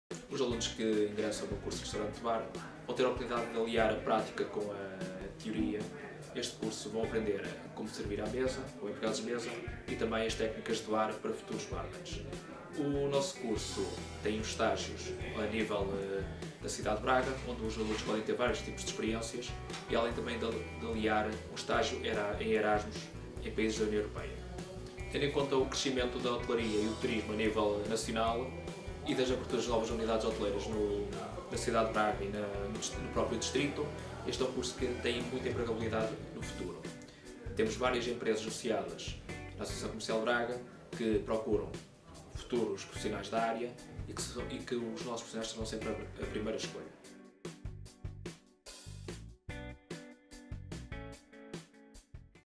Testemunho formador ACB
formação Tirar o Curso de Técnico(a) de Restaurante/Bar na ACB dá-te a oportunidade de fazer o 12.º ano com formadores de referência como o Luís Neves.